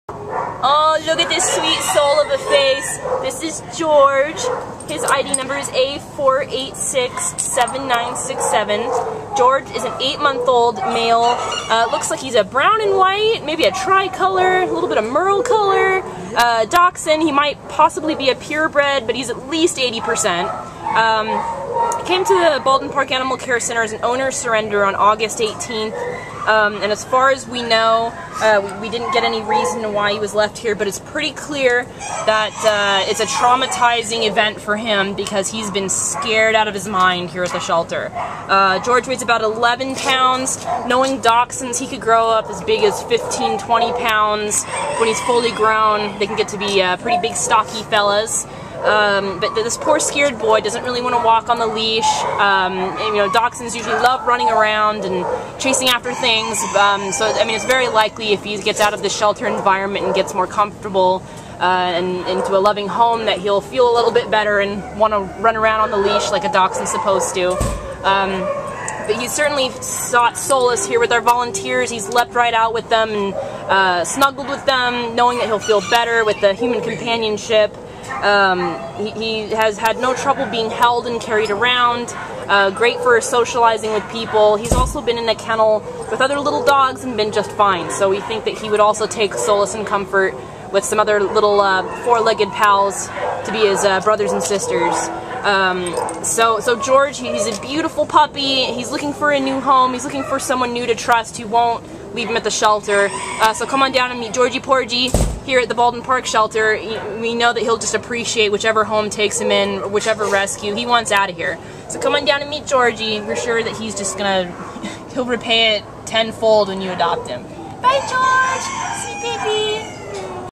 A4867967 George | Dachshund
A4867967 George is a shy-but-fly 8-month-old brown-and-white male Dachshund mix whose owners left him at the Baldwin Park Animal Care Center on August 18th. Weighing 11 lbs, George is a simply gorgeous baby who turns heads with each of his little steps! Unfortunately, this boy is quite scared in the shelter environment, and it is clear that he seeks the solace of humans. During our photo shoot, he walked right up to our volunteers for comfort. This is one guy who knows what he needs and wants! George is ready to ditch the shelter with his forever family and learn the training basics all young ones need to grow into happy and healthy adults. With his beautifully-speckled and spotted coat (that could make Jackson Pollock jealous!), George will be a fun and loving indoor pet for any active household that plans to include him in all of the day’s cuddle and play.